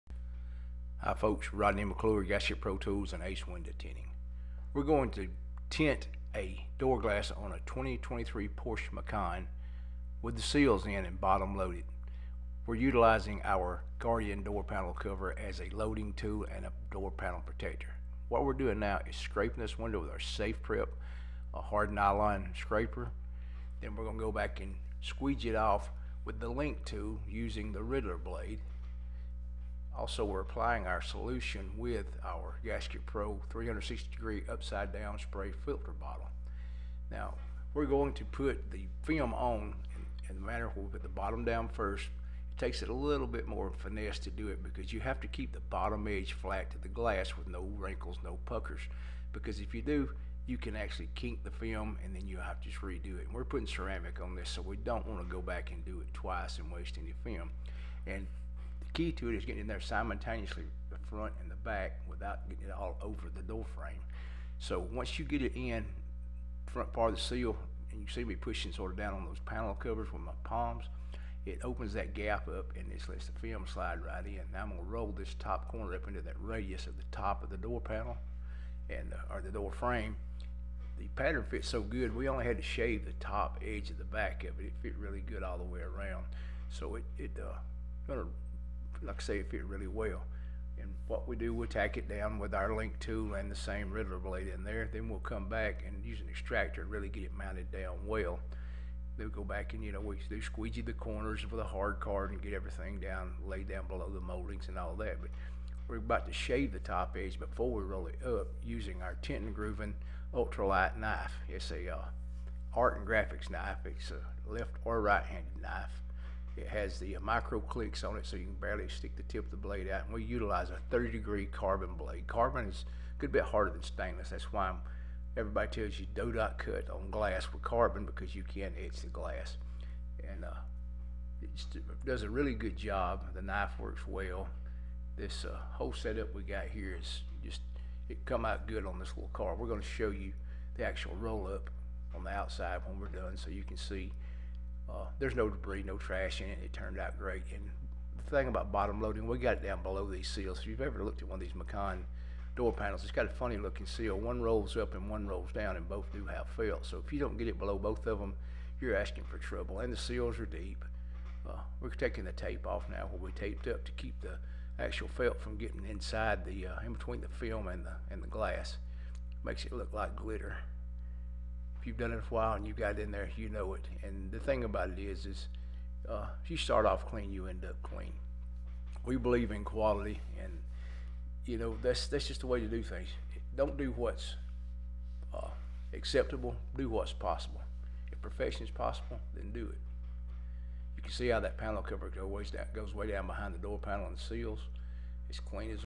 2023 Porsche Macan bottom load with seals in
We used Gasket Pro Tool's new Door panel cover as a panel protector and a film-loading tool to bottom-load this window with the seals in.